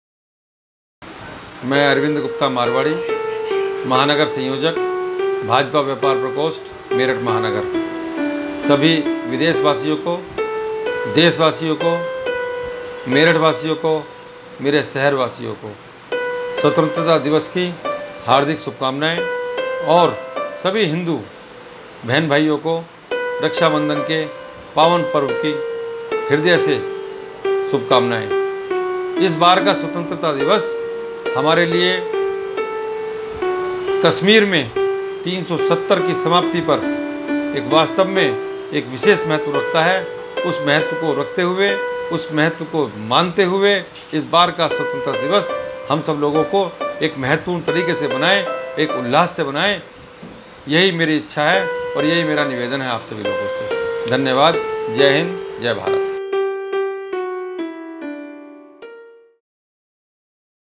अरविन्द गुप्ता मारवाड़ी, महानगर संयोजक भाजपा व्यापार प्रकोष्ठ मेरठ
अरविन्द गुप्ता मारवाड़ी, महानगर संयोजक भाजपा व्यापार प्रकोष्ठ मेरठ की तरफ से समस्त देश व प्रदेश वासियों को स्वतंत्रता दिवस और रक्षा बंधन की हार्दिक शुभकामनाएं